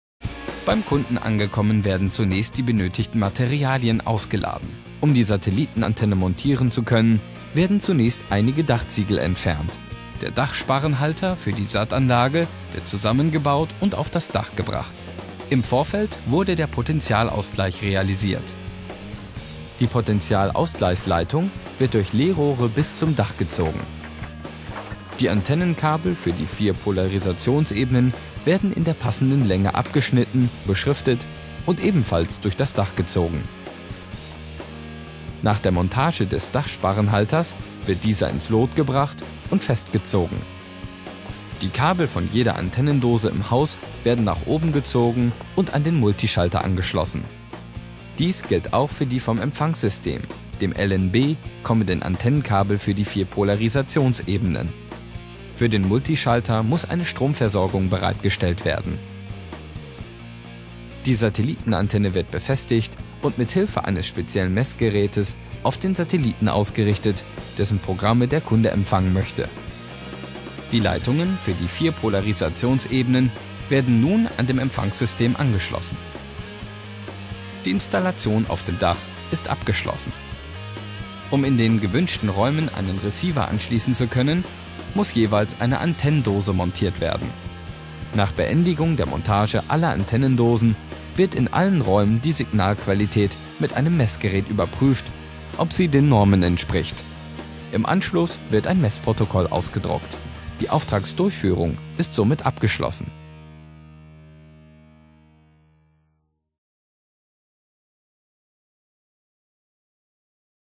Empfangsanlagen installieren - Auftragsdurchführung
Dieses Video zeigt die Auftragsdurchführung des Beruflichen Handlungsfeldes 4 der Kompetenzwerkst@tt Elektrohandwerk.
"Installieren und Inbetriebnehmen von Empfangs- und Verteilsystemen"